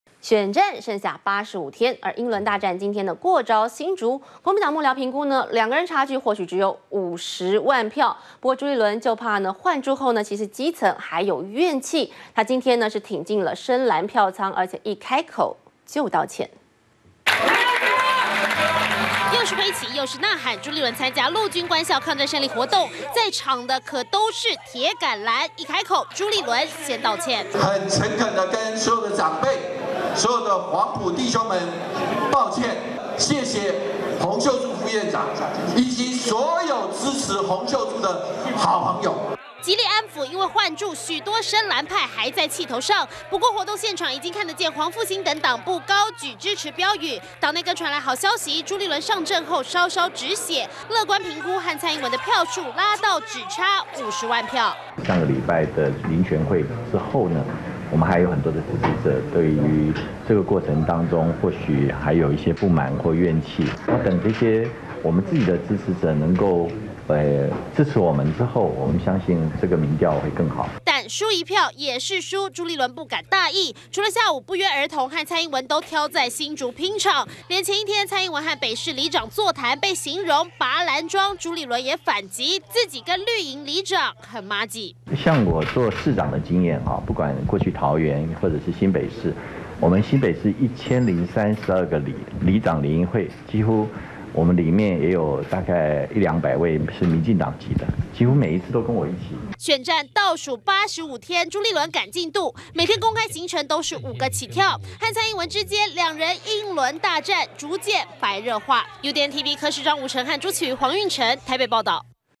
【2015.10.23】選情白熱化 英倫回防新竹較勁 -udn tv
選戰剩85天，英倫大戰今天過招，新竹國民黨幕僚評估兩人差距或許只有50萬票，不過朱立倫就怕換柱後基層還有怨氣，今天挺進深藍票倉，一開口就道歉。完整內容 , FB , Twitter , G+